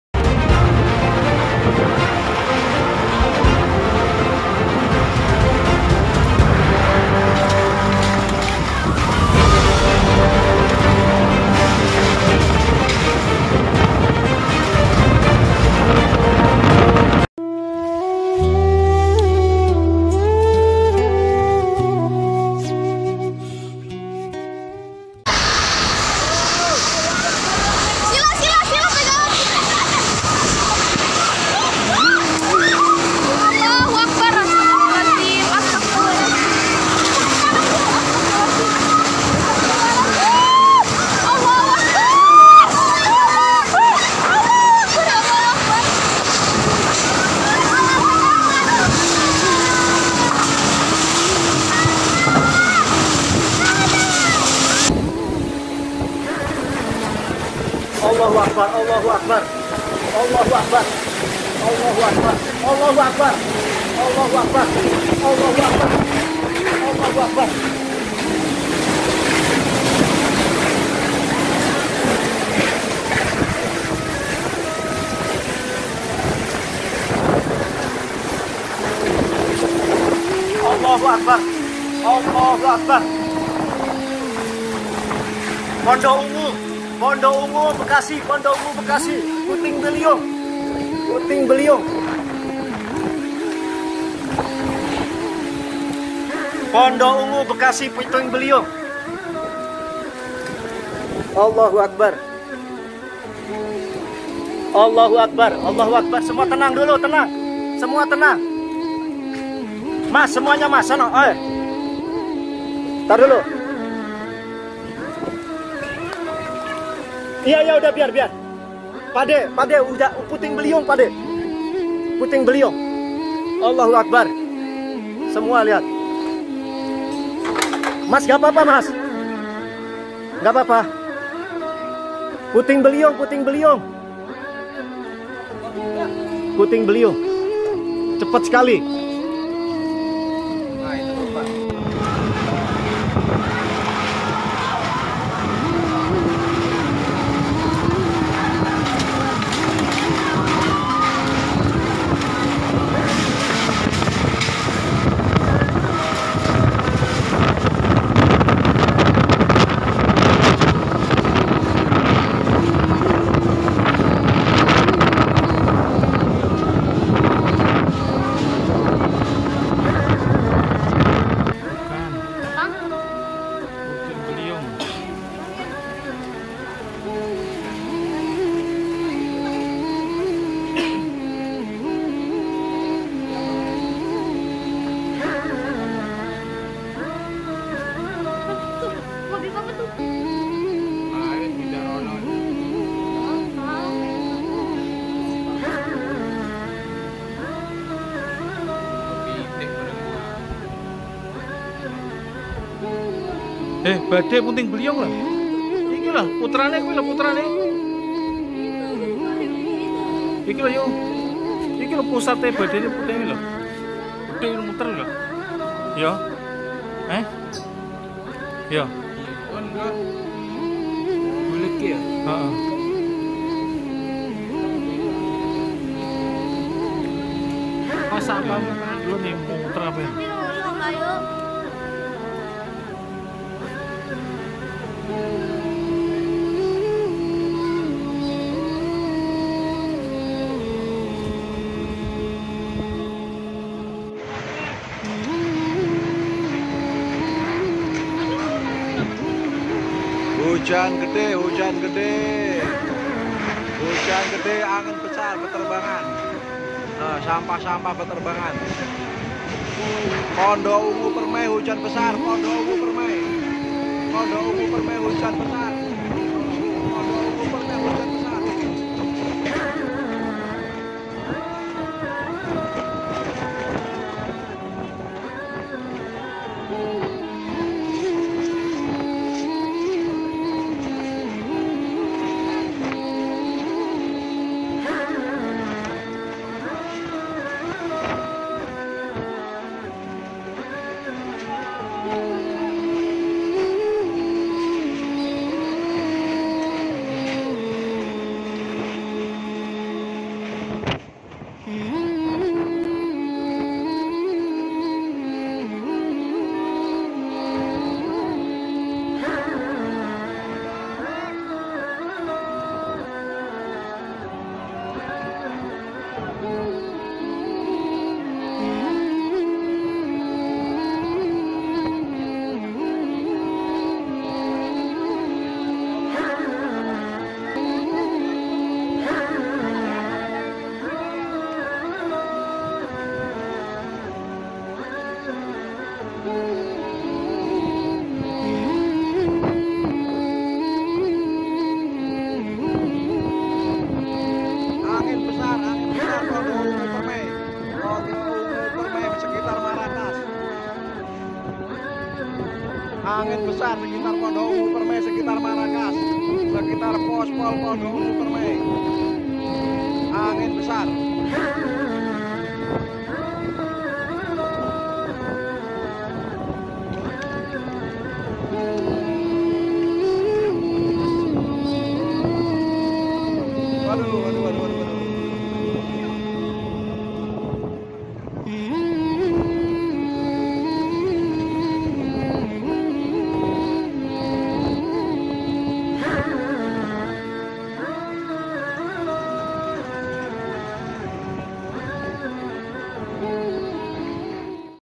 ANGIN PUTING BELIUNG DI PONDOK UNGU BEKASI
HARI INI 23 OKTOBER 2020 -  ANGIN PUTING BELIUNG TERJADI DI PONDOK UNGU BEKASI,
SMOGA KITA SMUA DI SELAMATKAN DARI SEGALA MACAM BENTUK BENCANA, MUSIBAH DAN FITNAH AKHIR ZAMAN INI.. AAMIIN YA ALLAH.. JAZAKALLAH KHAIR..